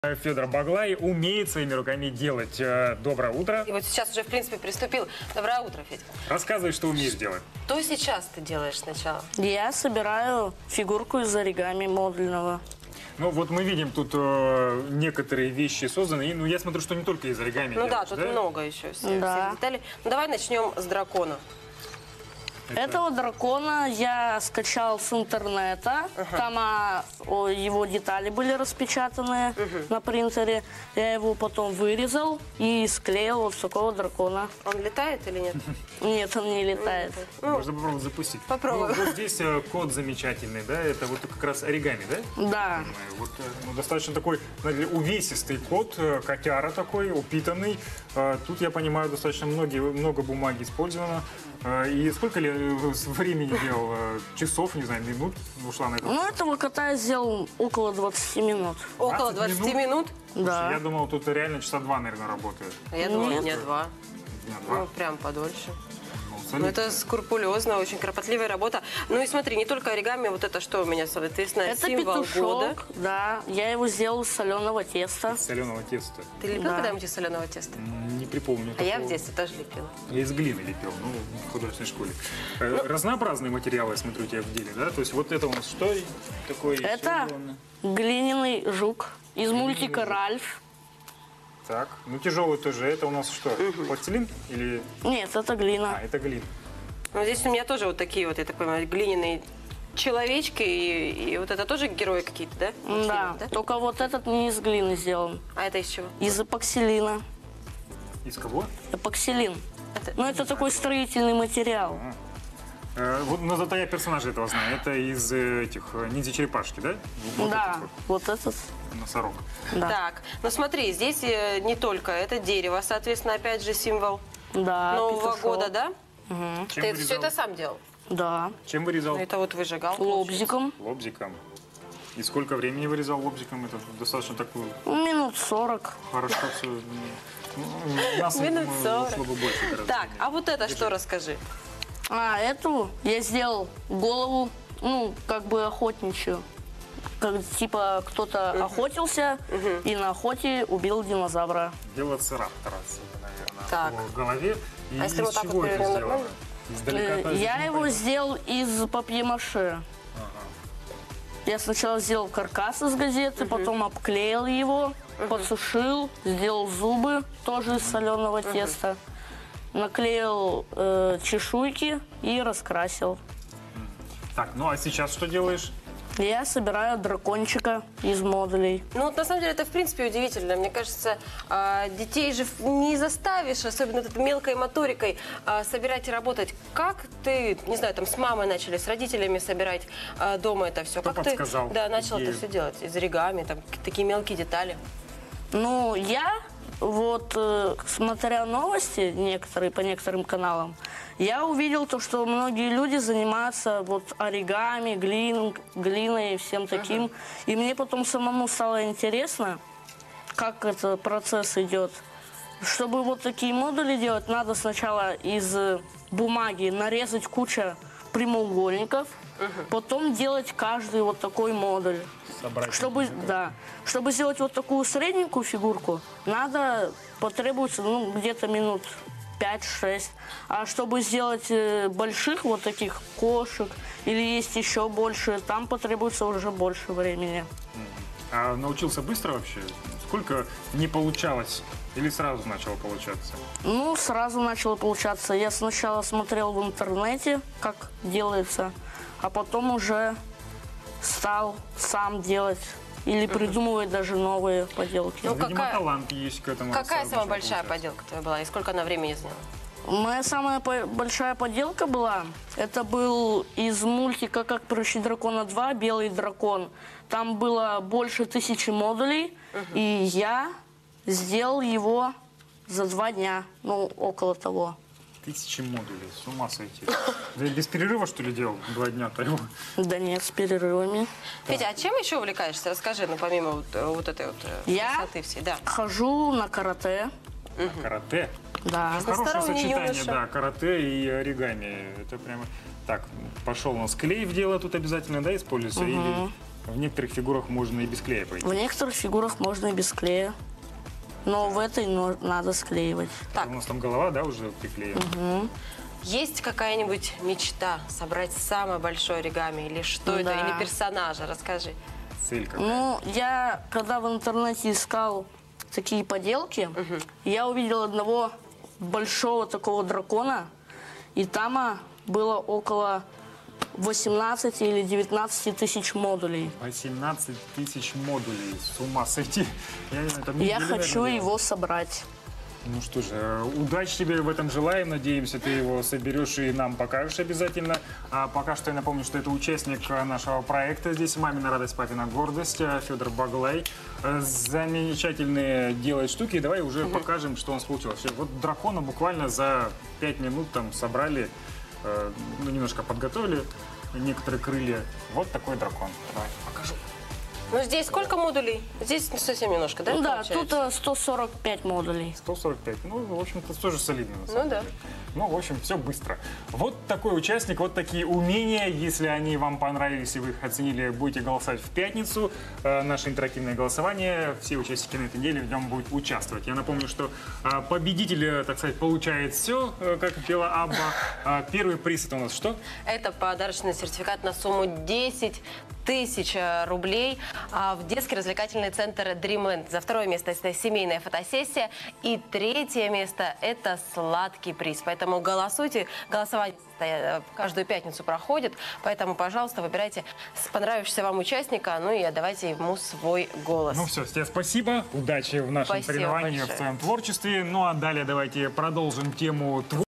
Как приручить дракона
Проект АФОНТОВО "Мамина радость, папина гордость"
Федор Баглай, занимается оригами